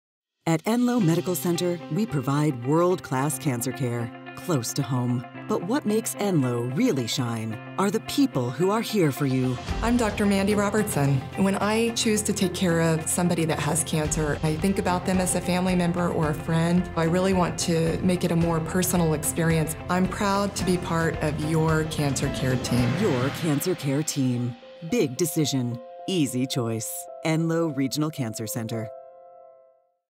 Meet Hematologist/Oncologist Mandy Robertson, M.D.
At Enloe Medical Center, we offer world-class cancer treatment, close to home. But what makes Enloe really shine are the people who are here for you, like hematologist/oncologist Mandy Robertson, M.D. Hear what drives her as part of your cancer care team.